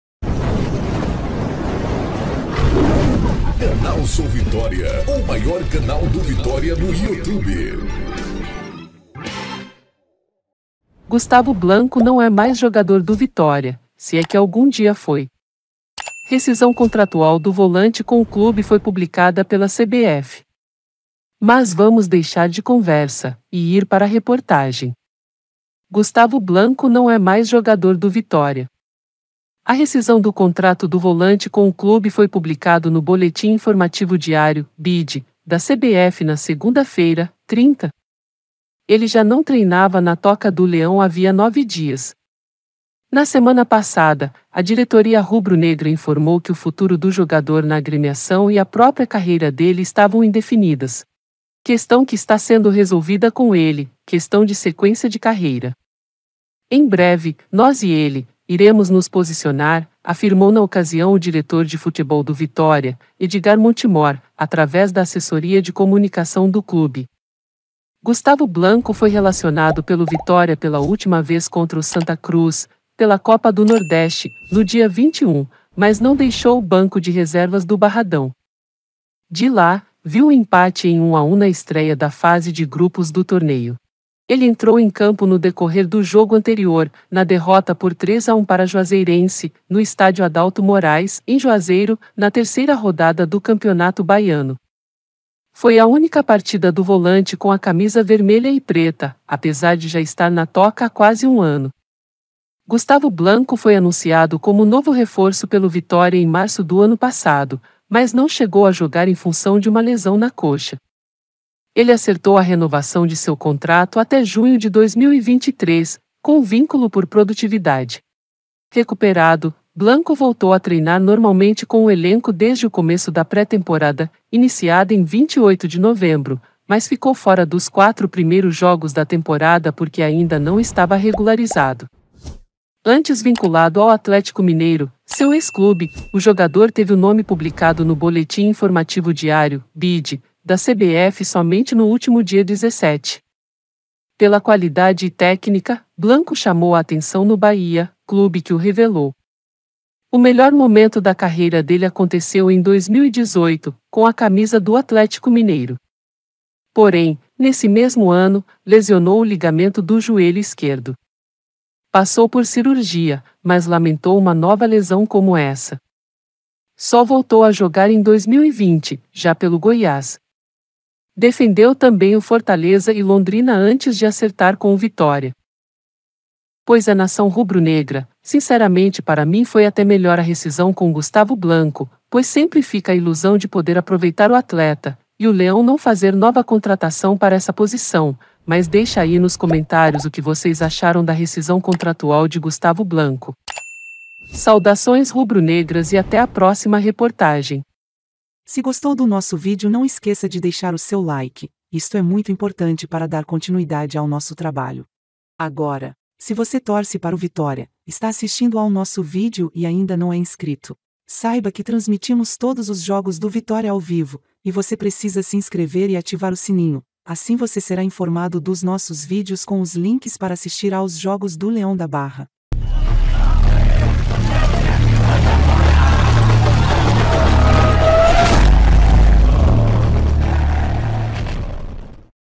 Volante não é mais jogador do Vitória
Rescisão contratual do volante Gustavo Blanco com o clube foi publicada pela CBF, Gustavo Blanco,
ecv
Gustavo Blanco não é mais jogador do Vitória. A rescisão do contrato do volante com o clube foi publicado no Boletim Informativo Diário (BID) da CBF na segunda-feira (30). Ele já não treinava na Toca do Leão havia nove dias. Na semana passada, a diretoria rubro-negra informou que o futuro do jogador na agremiação e a própria carreira dele estavam indefinidas.
"Questão que está sendo resolvida com ele, questão de sequência de carreira. Em breve, nós e ele iremos nos posicionar", afirmou na ocasião o diretor de futebol do Vitória, Edgard Montemor, através da assessoria de comunicação do clube.
Gustavo Blanco foi relacionado pelo Vitória pela última vez contra o Santa Cruz, pela Copa do Nordeste, no dia 21, mas não deixou o banco de reservas do Barradão. De lá, viu o empate em 1x1 na estreia da fase de grupos do torneio.
Ele entrou em campo no decorrer do jogo anterior, na derrota por 3x1 para a Juazeirense, no estádio Adauto Moraes, em Juazeiro, na terceira rodada do Campeonato Baiano. Foi a única partida do volante com a camisa vermelha e preta, apesar de já estar na Toca há quase um ano.
Gustavo Blanco foi anunciado como novo reforço pelo Vitória em março do ano passado, mas não chegou a jogar em função de uma lesão na coxa. Ele acertou a renovação de seu contrato até junho de 2023, com vínculo por produtividade.
Recuperado, Blanco voltou a treinar normalmente com o elenco desde o começo da pré-temporada, iniciada em 28 de novembro, mas ficou fora dos quatro primeiro jogos da temporada porque ainda não estava regularizado. Antes vinculado ao Atlético-MG, seu ex-clube, o jogador teve o nome publicado no Boletim Informativo Diário (BID) da CBF somente no último dia 17.
Pela qualidade e técnica, Blanco chamou a atenção no Bahia, clube que o revelou. O melhor momento da carreira dele aconteceu em 2018, com a camisa do Atlético-MG. Porém, nesse mesmo ano, lesionou o ligamento do joelho esquerdo. Passou por cirurgia, mas lamentou uma nova lesão como essa. Só voltou a jogar em 2020, já pelo Goiás. Defendeu também o Fortaleza e Londrina antes de acertar com o Vitória.

ecv